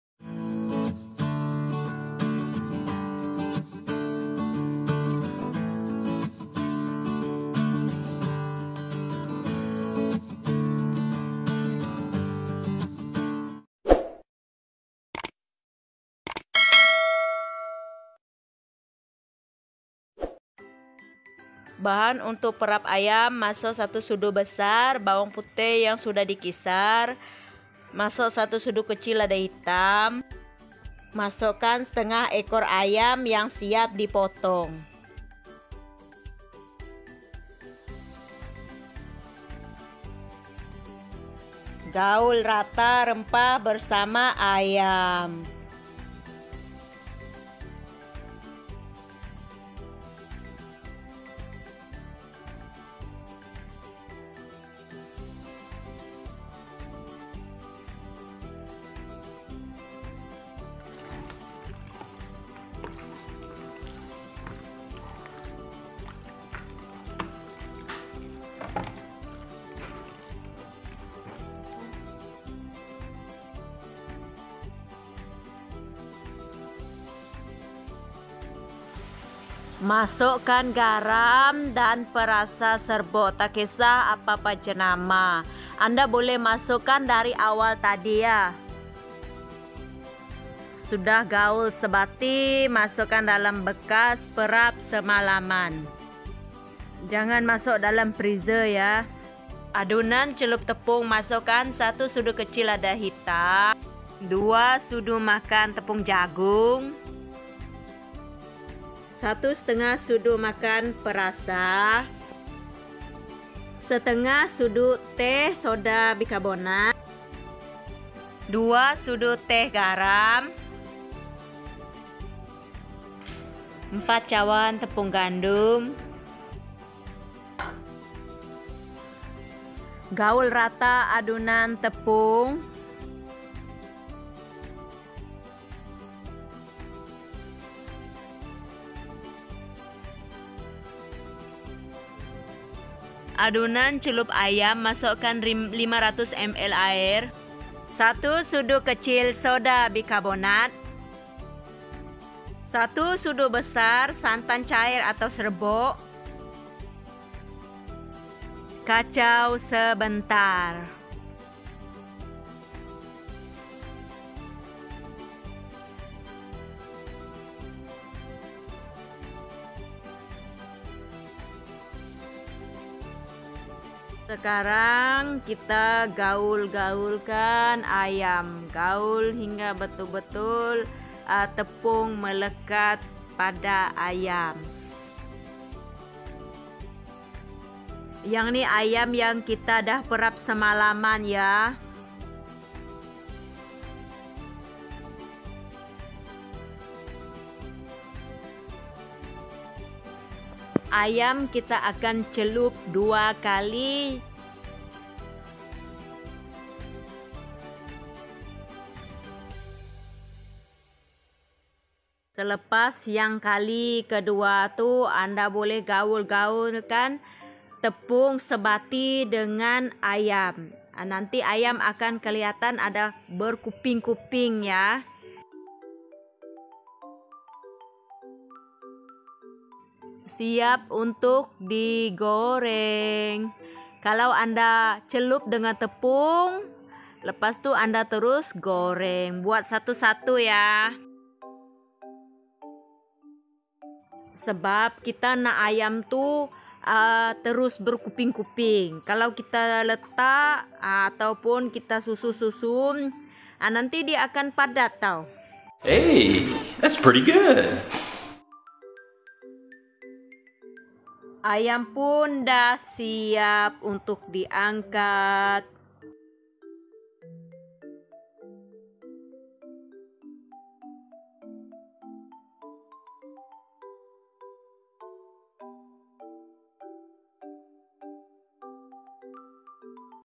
Ayam Goreng Crispy ala KFC|Kriuk!Kriuk|gurih
Ayam goreng Crispy ala kfc,sememangnya jadi kegemaran didunia,namun sebenarnya kita boleh mengolahnya sendiri,cuma perlukan kasih sayang diserlahkan dalam masakan😊walaupun tidak 100 peratus sama.Jom kita pratikkan Ayam goreng Crispy ala kfc.

Setengah ekor ayam
1 sudu kecil Lada hitam
1 sudu kecil Serbuk perasa
 1 sudu kecil Bawang kisar
1 sudu kecil garam

Bahan adunan tepung celup:
4 cawan tepung gandum
1 1/2 sudu besar perasa Knorr pasti sedap
1/2 sudu kecil soda bikarbonat
2 sudu kecil kecil garam
1 sudu kecil lada hitam

Bahan adunan air untuk dicelup:
500ml air sejuk
1 sudu besar santan cair @serbuk
1 sudu kecil soda bikarbonat

Simak videonya..